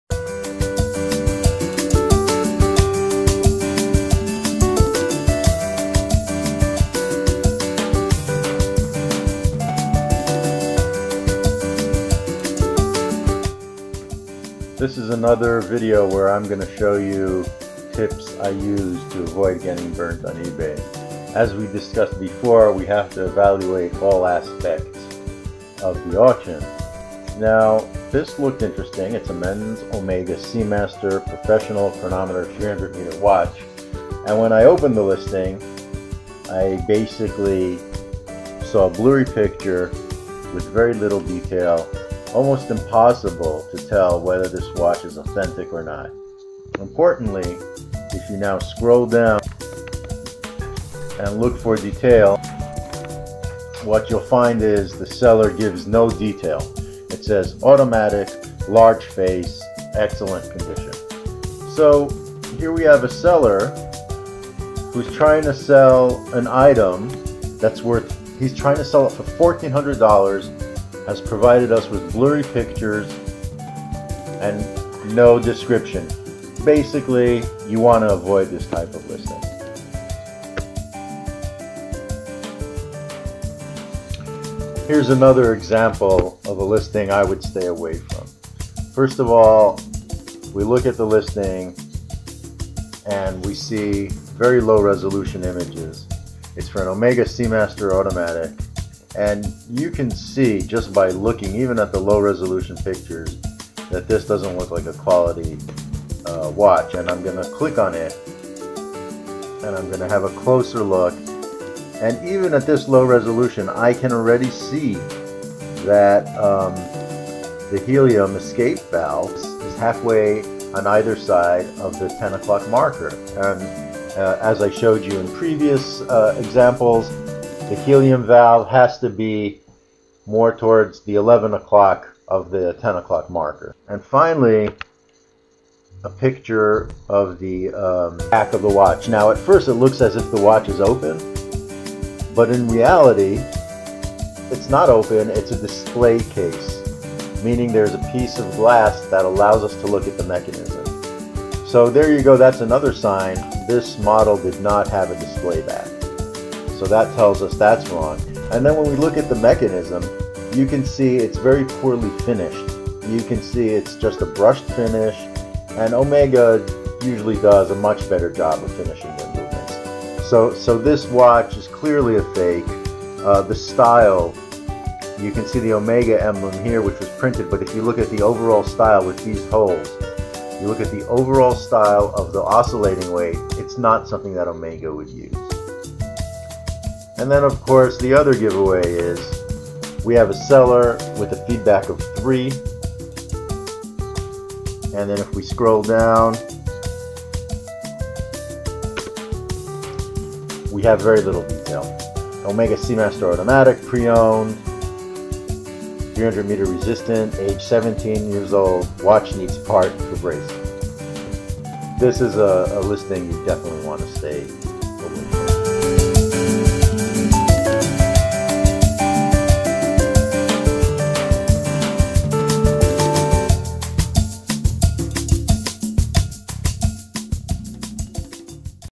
Replica Omega Watch Detection
A second video in the series of tips for detecting fake or replica Omega watch listings. Replica Omega watch listings detection - the video gives examples of ebay listings you would like to avoid.